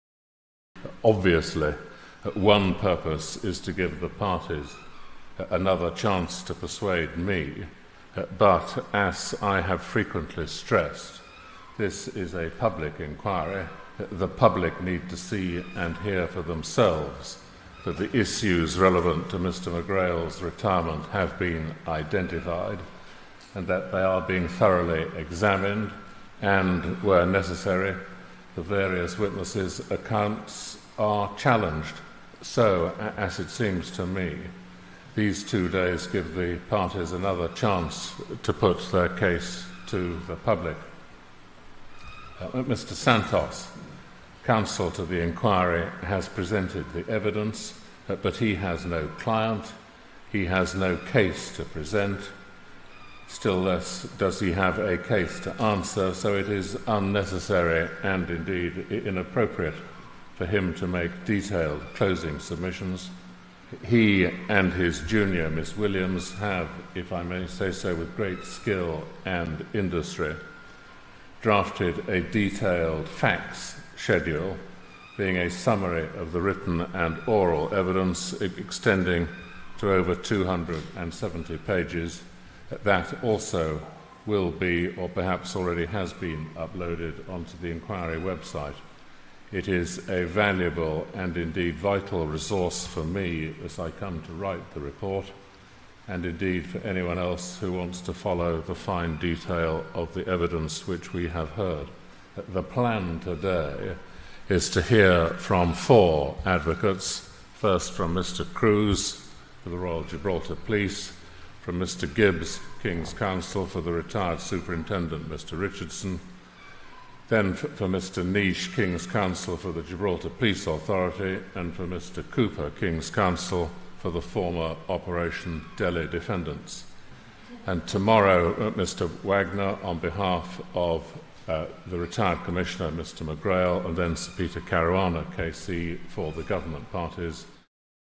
Inquiry Chairman introduces closing arguments
What are the McGrail Inquiry closing submissions about? Chairman Sir Peter Openshaw says it’s to make sure the public sees the issues relevant to Mr McGrail’s retirement have been identified, thoroughly examined and where appropriate accounts challenged: one final chance for the core participants to put their cases to the public.